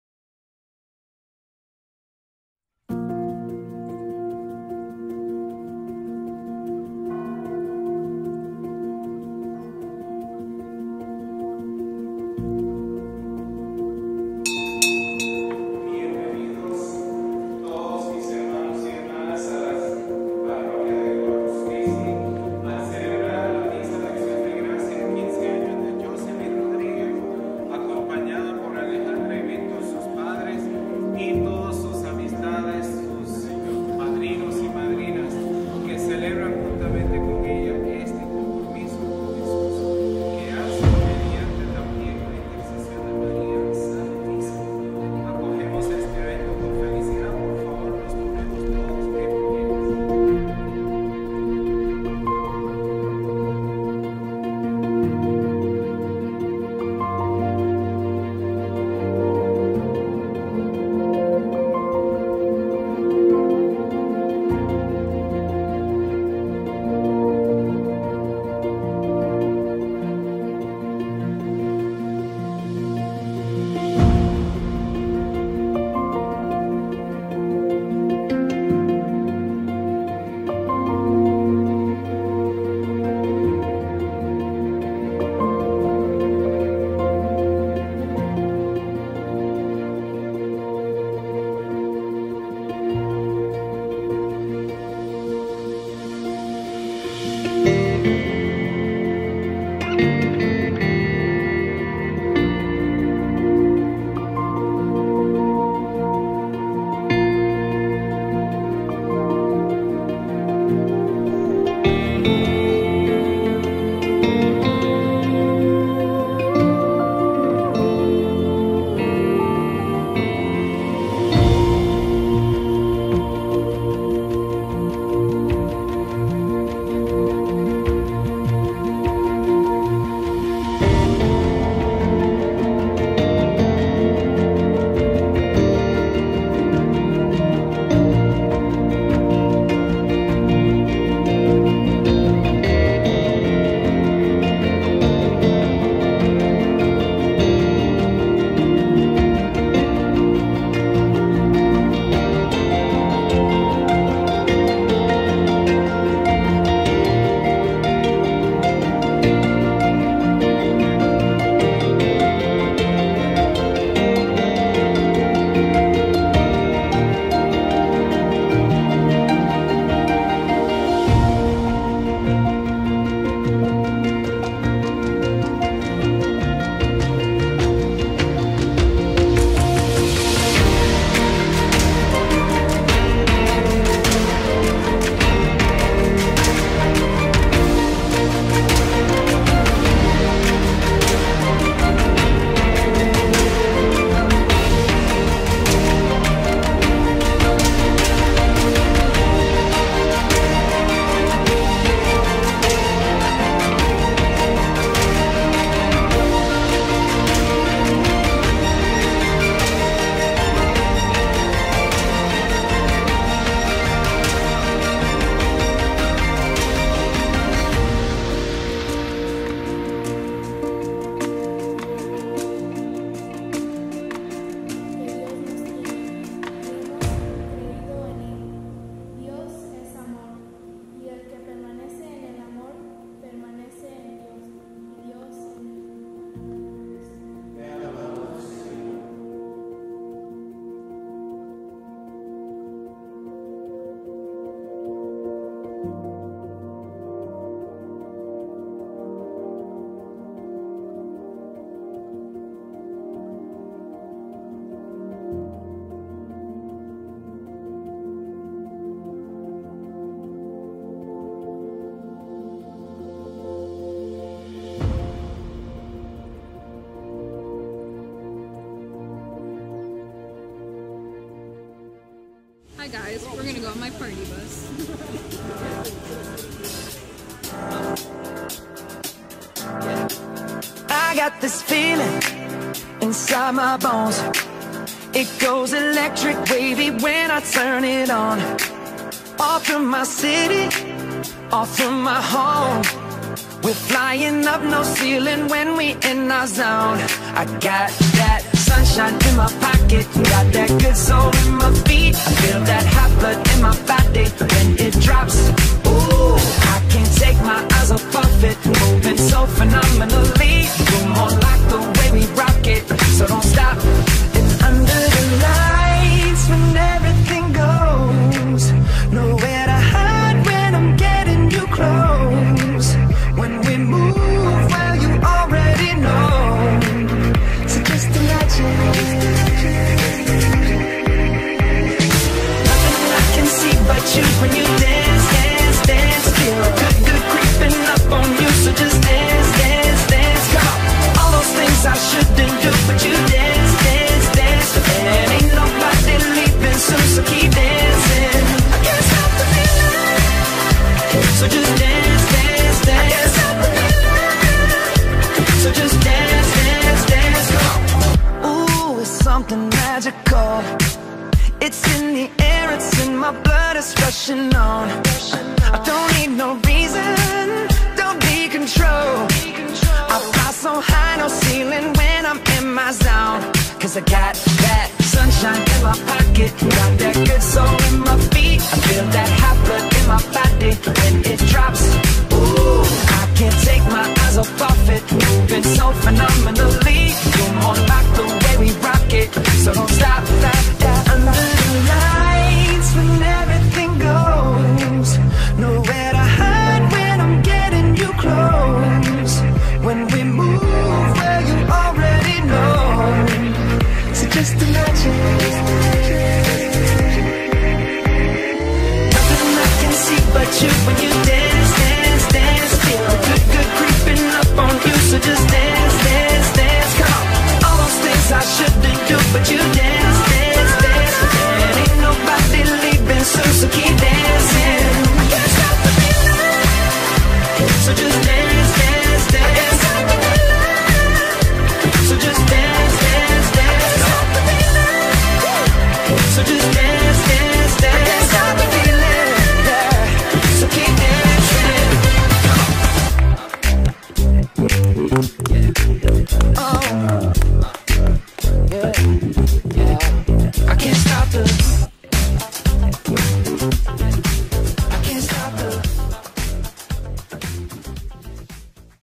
The Ceremony of Youth | Quinceañera
The wonderful joy to be able to capture these special moments is priceless!

Always happy to be part of that special day! :)